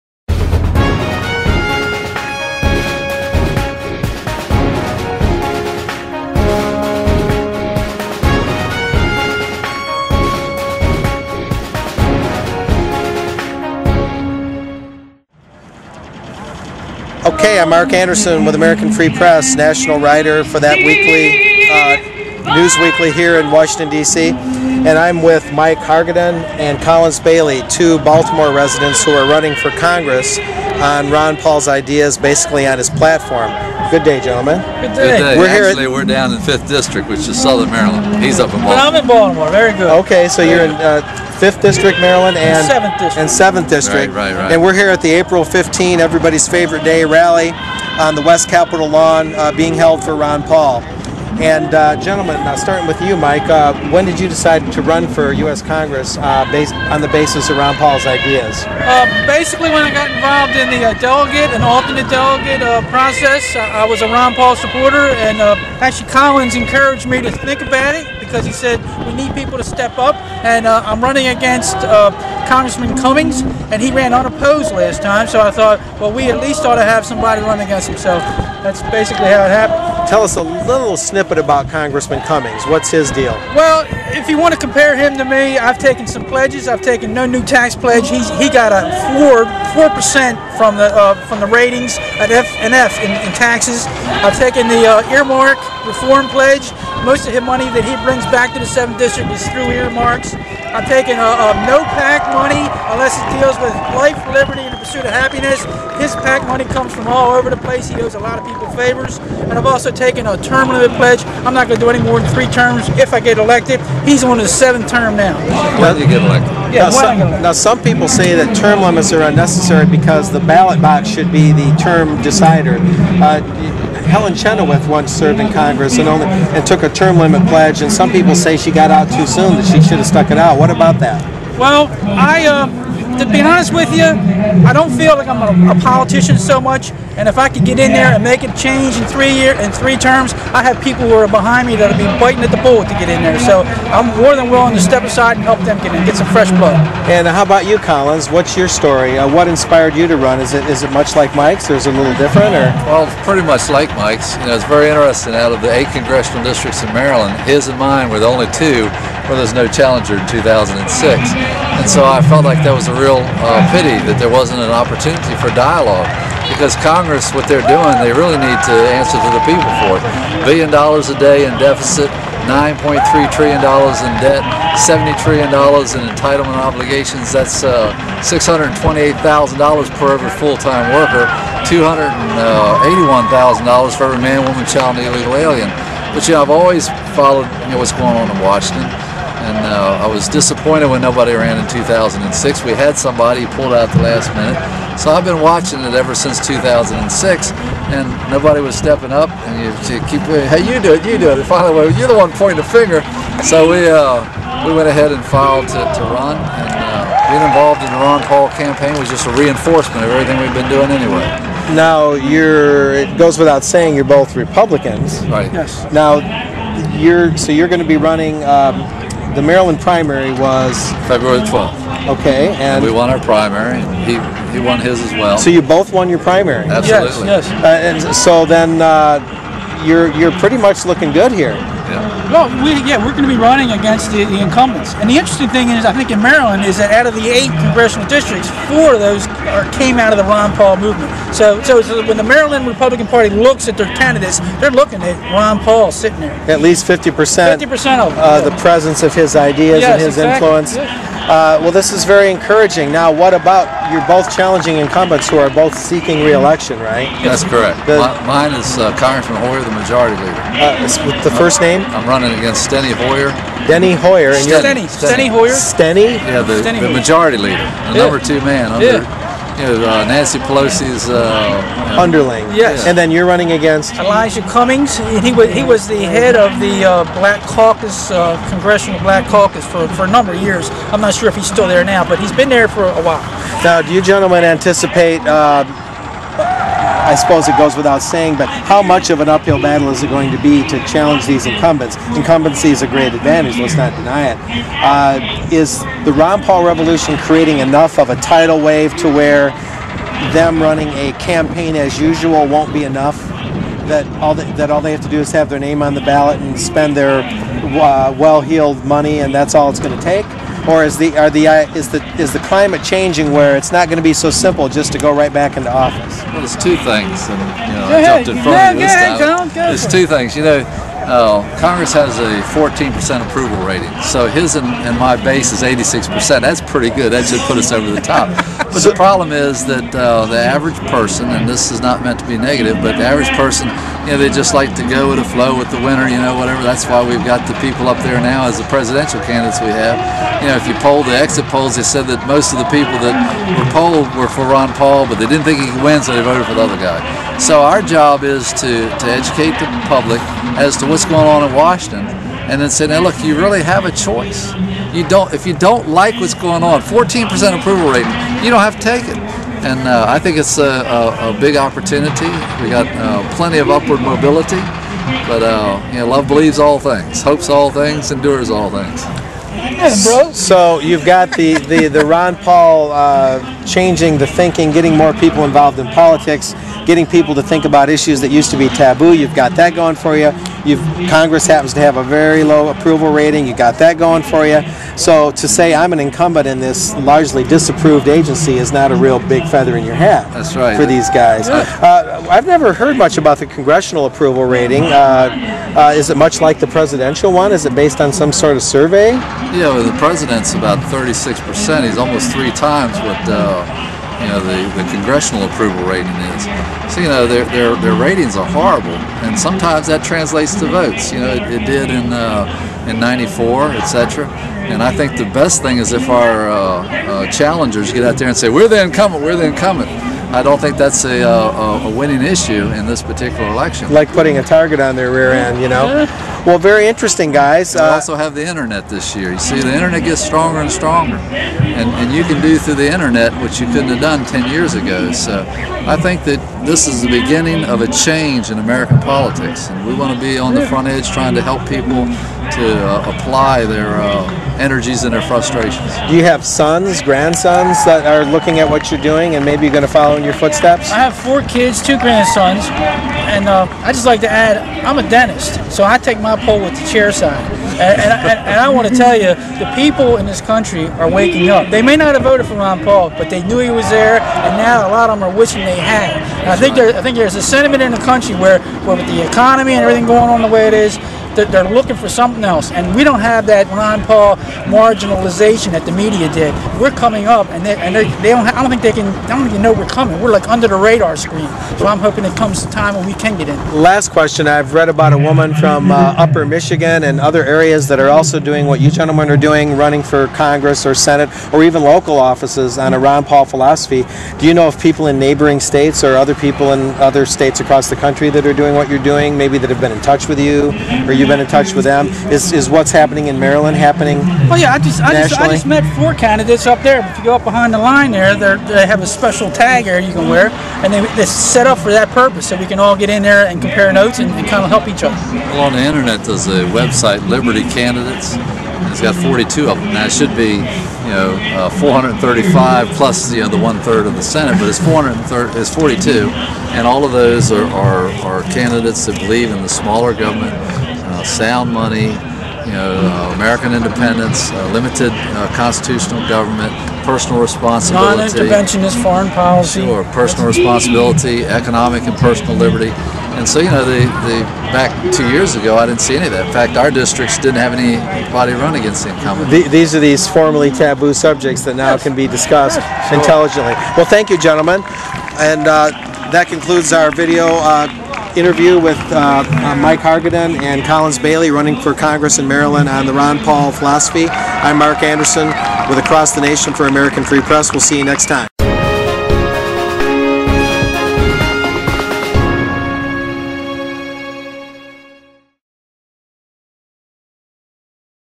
Ron-Paul-Style Republicans Run for Congress
Americanfreepress.net correspondent Mark Anderson interviewed congressional candidates Mike Hargadon and Collins Bailey, who are both running in Maryland on platforms similar to Ron Paul's against incumbents in the U.S. Congress.

to get books and information not available elsewhere.